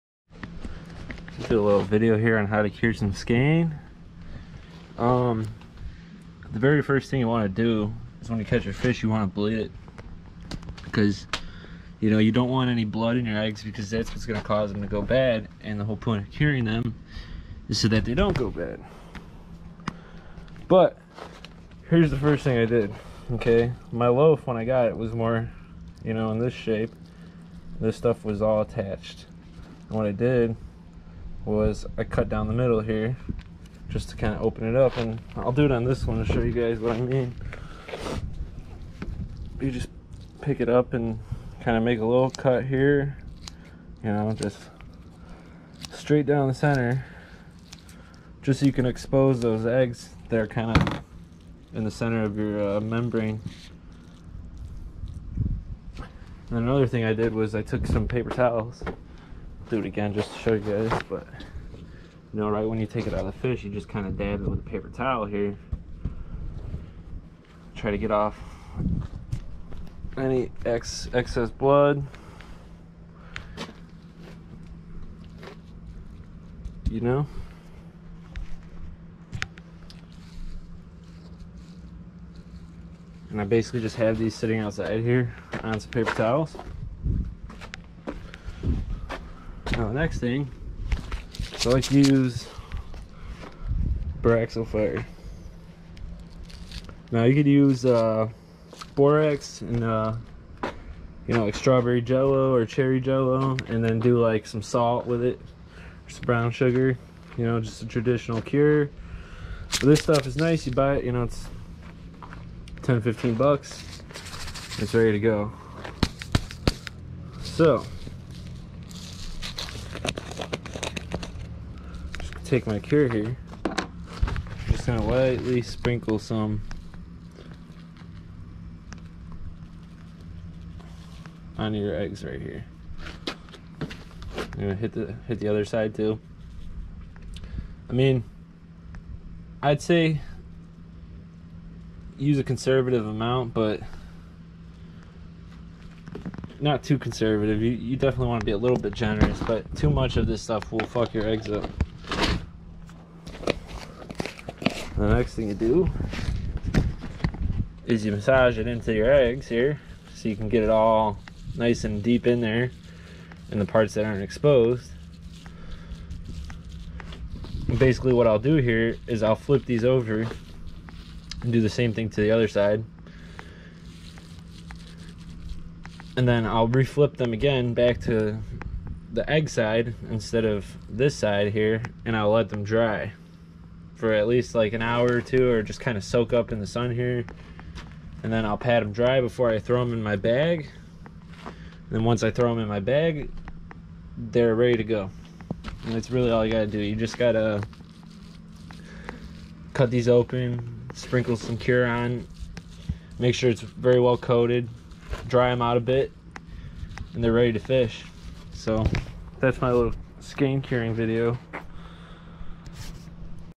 How To Cure Salmon Skein Easy
Jeremy Trout Showing us his secrete recipe to cure Salmon Skein to Catch Monster Fish